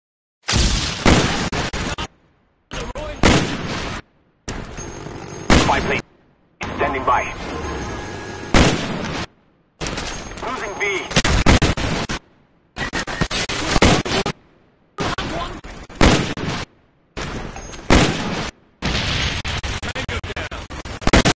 JAMES171992 - sniper kills
about 7-9 sniper kills not to sure all are hs but i dont care i get the
kill so dont cry about it